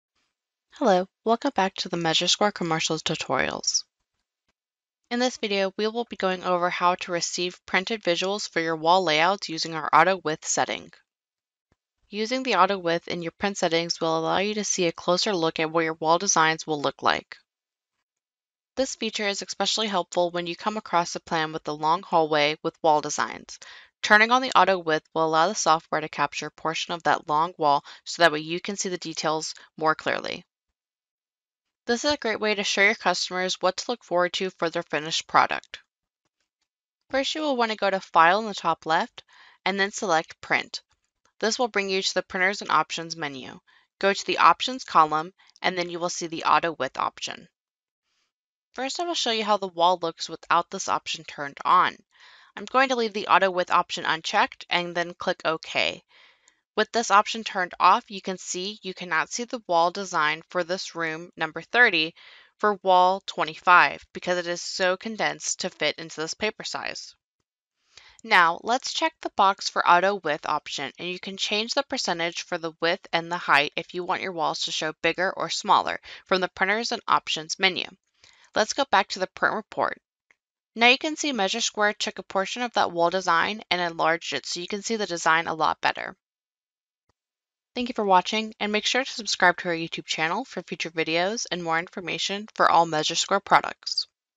Print Wall Profiles on Print Report - MeasureSquare 8
In this video we will be going over how to receive printed visuals of your wall layouts using our ‘Auto Width’ setting. Using the ‘Auto Width’ in your print settings will allow you to see a closer look at what your wall designs will look like